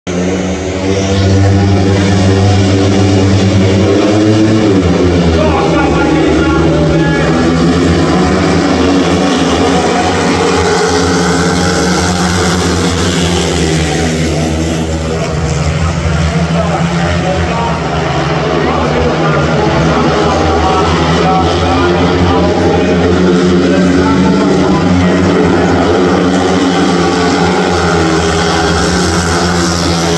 Landshut Seemann Pokal Lauf 20 Sturz 22.08.2024
rot Wassermann
blau Hillebrand
weiß Bowes
gelb Kössler